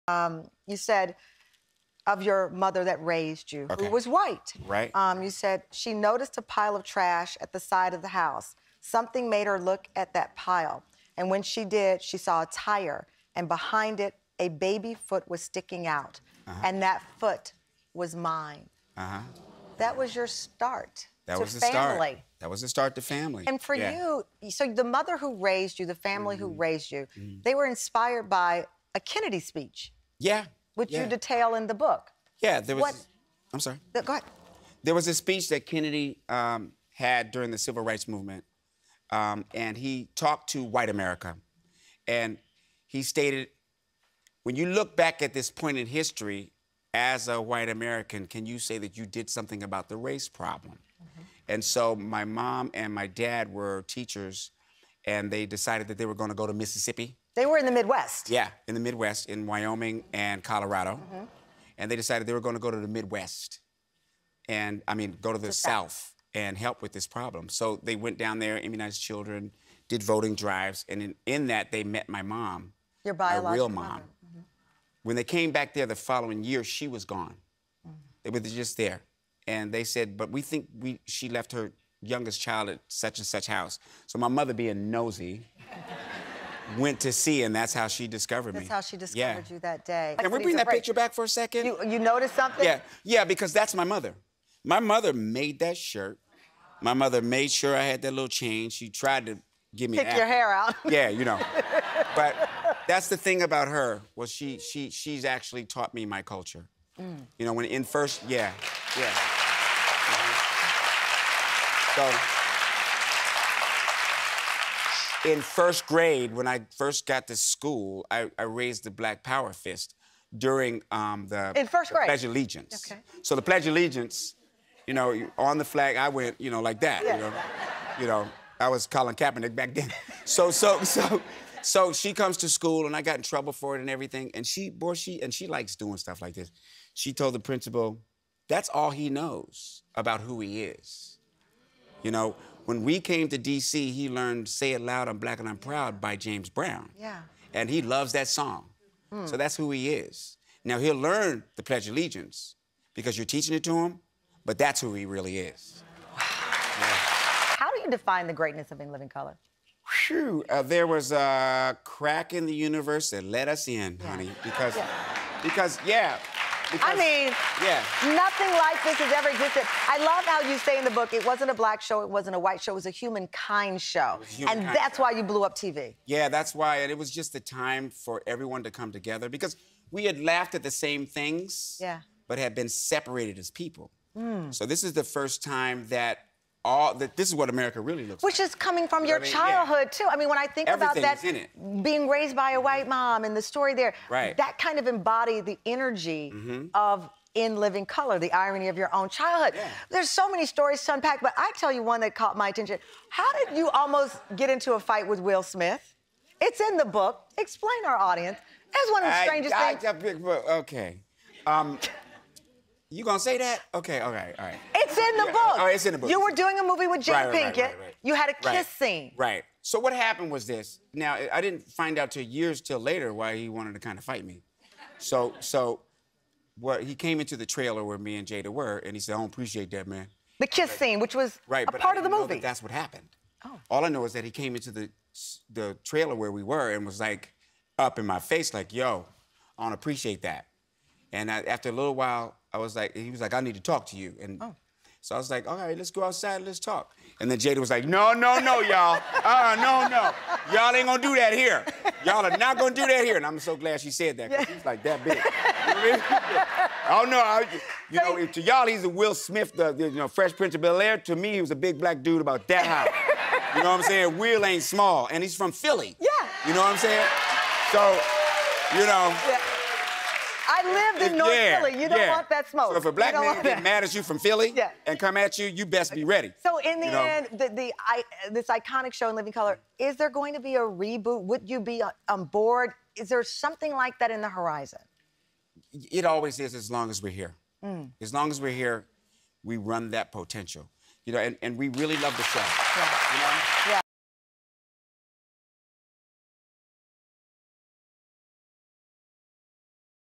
Actor & Comedian Tommy Davidson On Why Will Smith Wanted To Fight Him
Funnyman and actor Tommy Davidson dishes on new book, being raised by a white family and if there will be a “In Living Color” reboot.